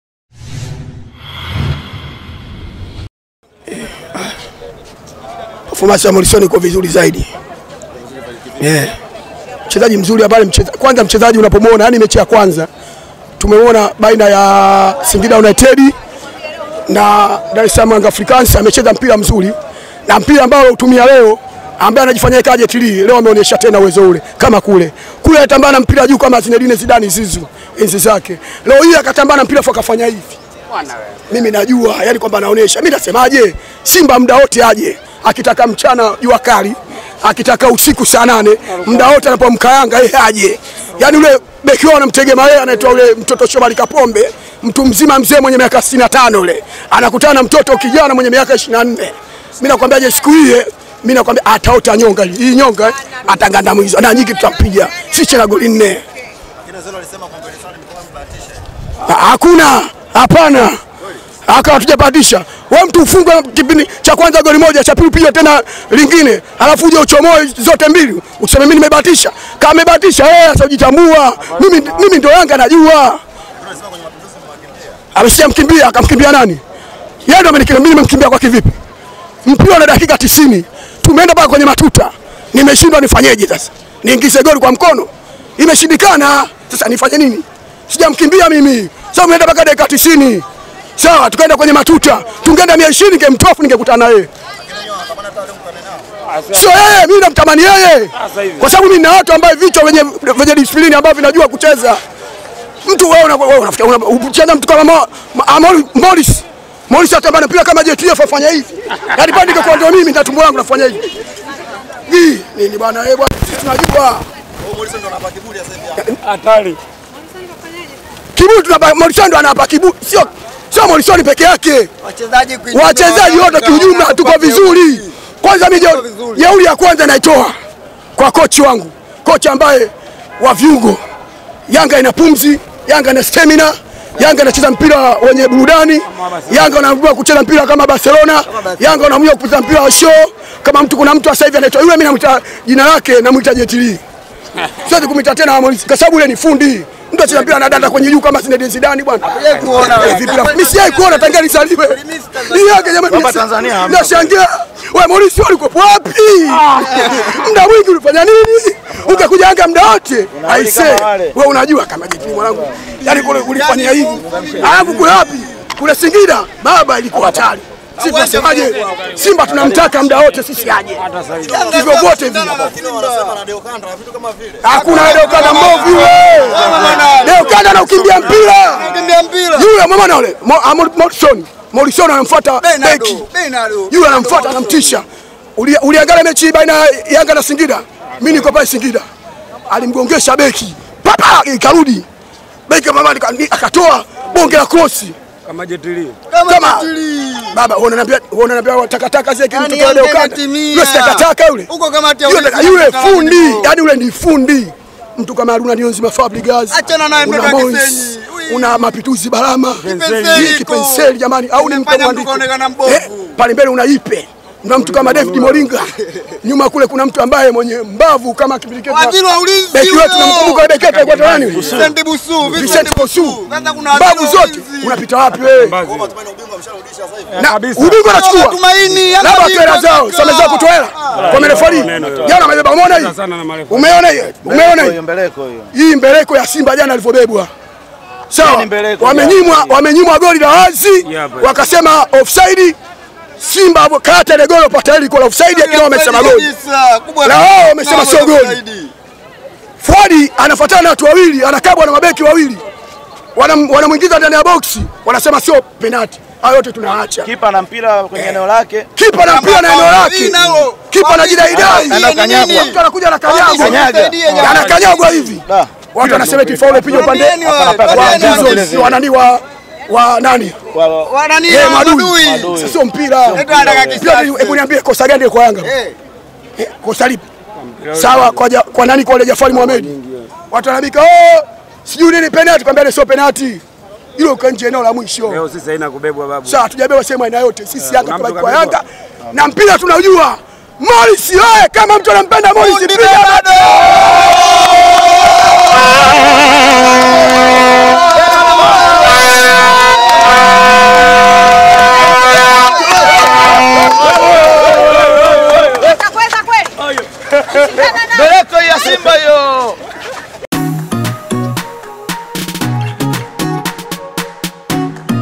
Shabiki YANGA: Morrison Mtamu Mpaka Anakera 'Kama Jet li'
TIMU ya Yanga SC imeungana na vigogo wenzao, Simba SC kutinga hatua ya 16 Bora Kombe la Shirikisho la Soka Tanzania (TFF), maarufu kama Azam Sports Federation Cup (ASFC) baada ya ushindi wa mabao 2-0 dhidi ya Tanzania Prisons jioni ya leo Uwanja wa Taifa, Dar es Salaam.